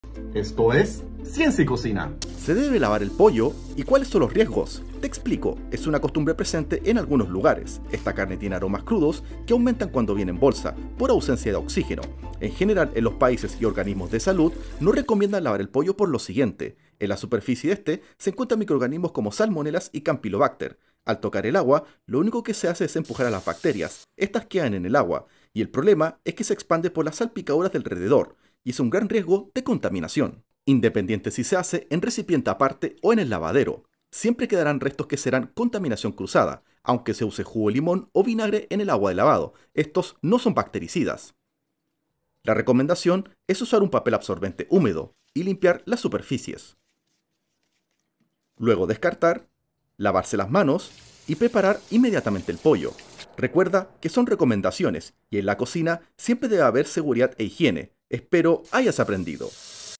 ¿Se debe lavar el Pollo?
Una pregunta común de muchos, ¿será necesario lavar el pollo?, aquí te explico los riesgos y como te recomiendo hacerlo. Aplica a todo lavado de carnes en general